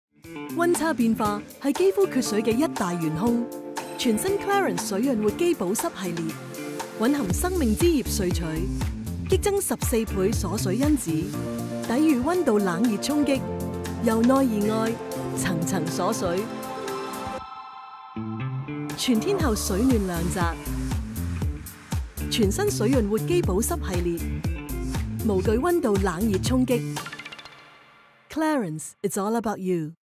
CLARINS 全新水潤活肌保濕系列 無懼溫度冷熱衝擊 全天候水嫩亮澤
CLARINS 全新水潤活肌保濕系列
無懼溫度冷熱衝擊 全天候水嫩亮澤

您知道溫差變化是令肌膚乾燥缺水的一大元兇嗎？

CLARINS科研團隊一直竭力解開令肌膚乾燥缺水的奧秘，突破性發現冷熱衝擊是重要關鍵 ——
肌膚海綿鎖水因子在溫度急劇變化下倍速銳減，水分瞬間大量流失。城市人每日需承受多達17次的冷熱衝擊，
鎖水力不足的肌膚變得脆弱乾燥及失去光澤。

為強效激活肌膚鎖水保濕力，CLARINS突創全新水潤活肌保濕系列，
蘊含高效神奇植物有機生命之葉 (Leaf of Life) 萃取，讓肌膚內的海綿鎖水因子以14倍激增，層層重啟鎖水效能，
日夜抵禦溫差變化，由內而外修復肌膚天然保濕屏障，肌膚持久水嫩，重煥光澤。

從此無懼冷熱頻襲，全天候綻放水潤光采。